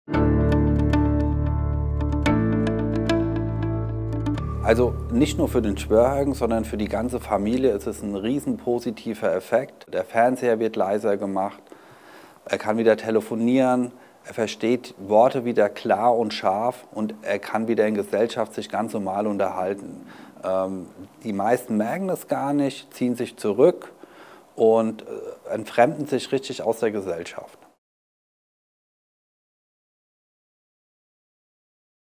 Wir fragen einen Hörakustiker: Welche Vorteile haben Hörgeräte?
Wie verändert sich mein Leben durch Hörgeräte? Und welche Vorteile bringt ein Hörsystem für mich und mein Umfeld? Wir haben einen Hörakustiker zu den wichtigsten Fragen rund um die Themen Hören, Hörtest und Hörgeräte befragt. Danke @Hörgeräte Collofong für die Unterstützung bei den Dreharbeiten.